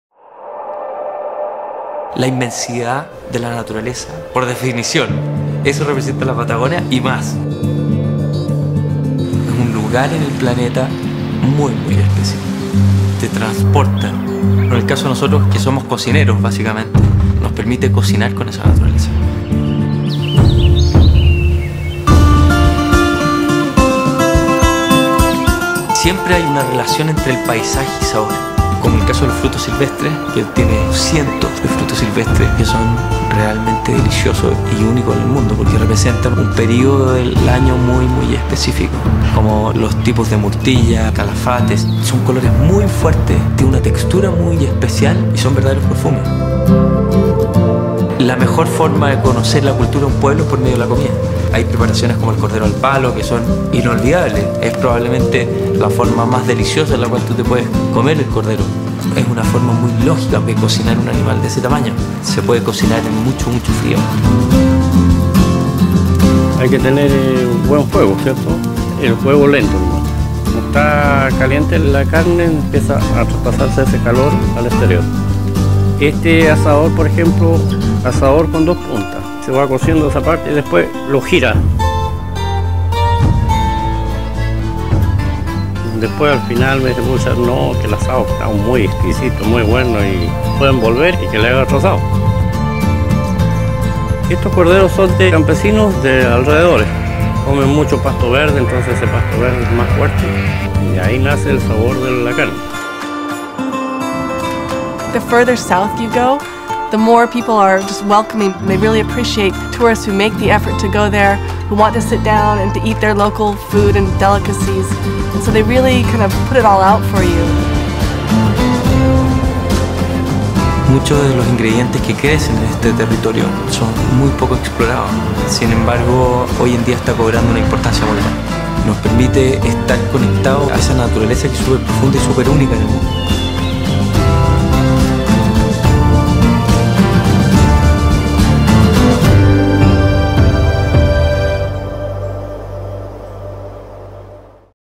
Cosa fare in Cile: Gastronomia Patagonia - Natura Aperta
La natura indomita della Patagonia offre ingredienti unici che portano questo spirito servaggio ad ogni piatto. Uno di essi è il famosissimo cordero al plato, ricetta leggendaria di questa zona del nostro paese, perfetta per gli amanti della buona tavola. Ti invitiamo a scoprire sapori esotici ed unici nel luogo più australe del nostro paese.